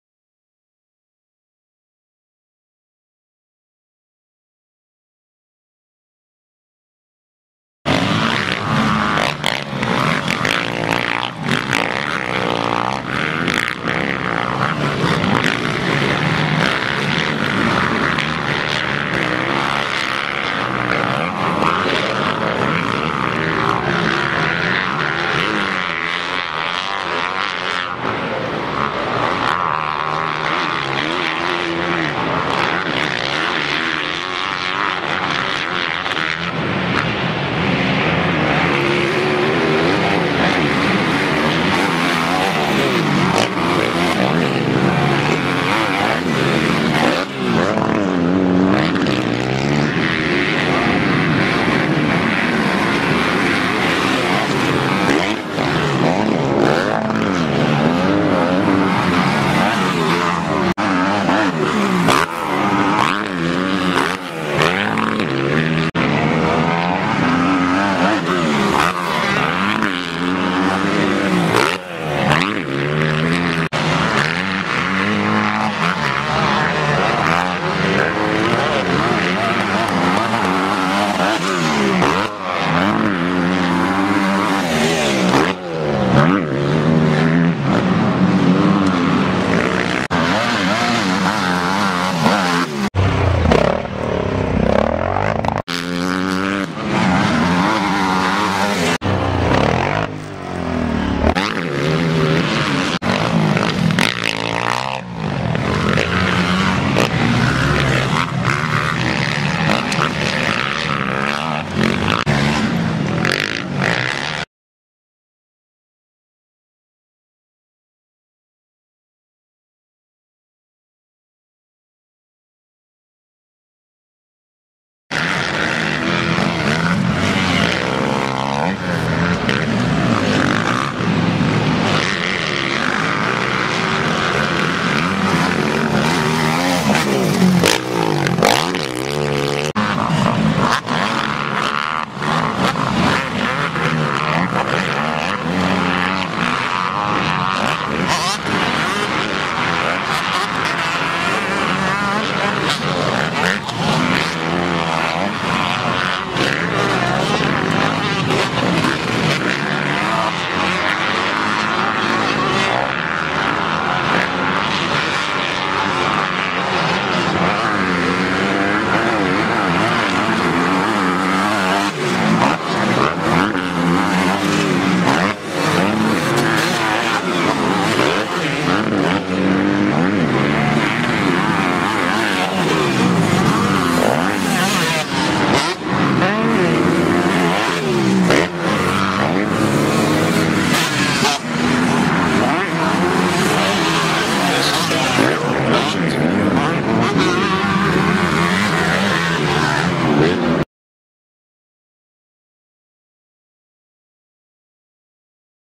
John Dowd 7-31-10
Pro AMA Motocross rider John Dowd racing at Hemonds Motocross Park in Minot Maine. i was working the finish line jump so here are some of the shots